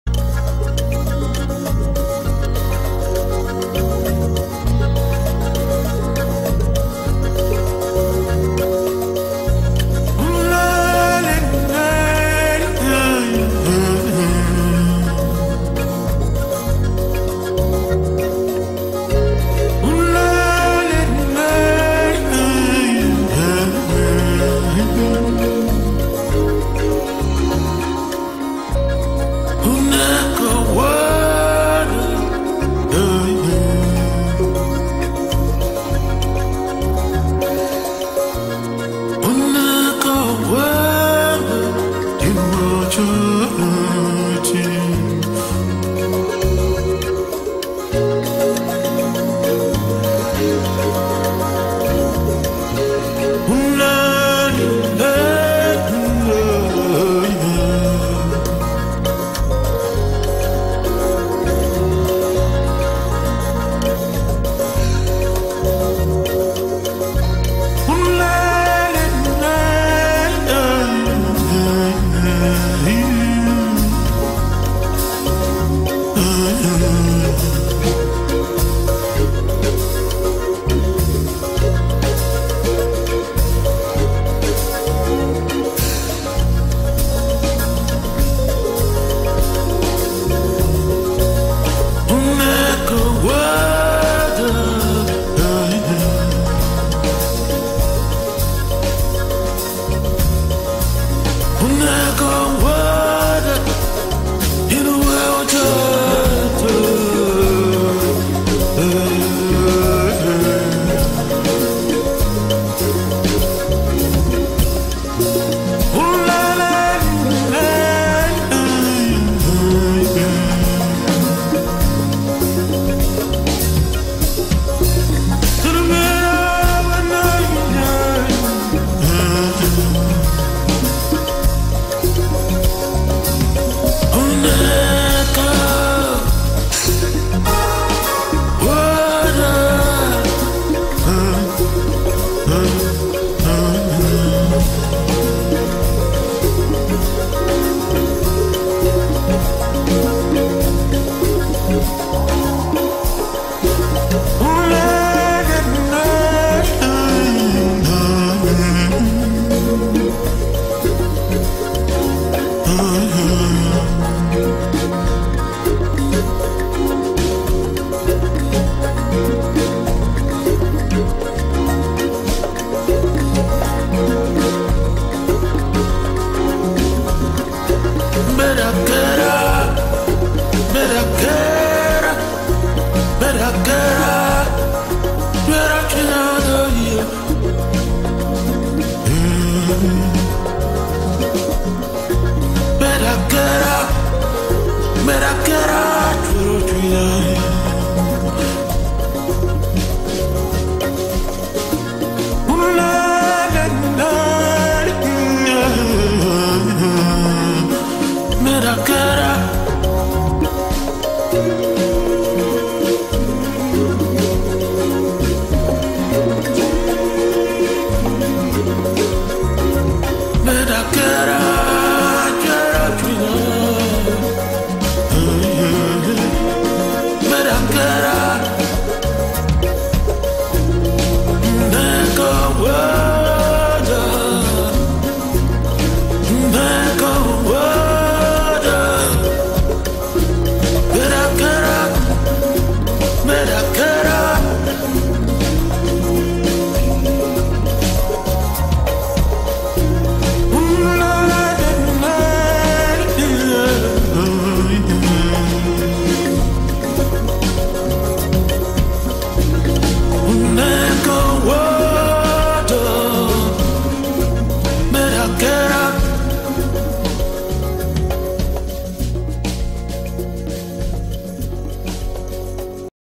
Morelia Viridis Merauke (mue/shedding).
Je vous présente un montage vidéo (HD) personnel de mon viridis localité Merauke âgé d'un an en train de retirer sa mue.
Mue parfaite de 120cm (mue abimée au niveau de la tête).